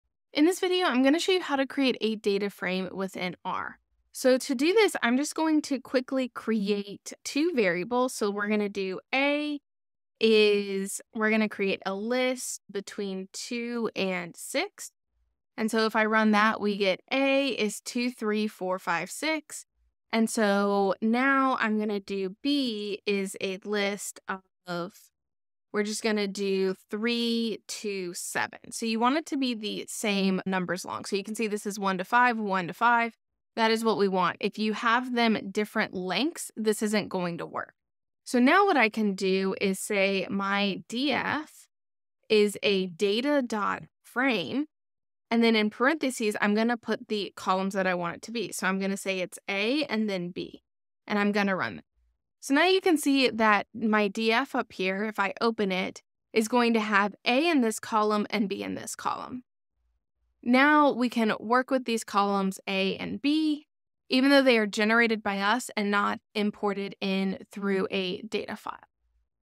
How to Create A Data Frame in R using RStudio | Data Analysis in R for Beginners
In this video, I will show you how to create a dataframe of data in R and use R built-in datasets.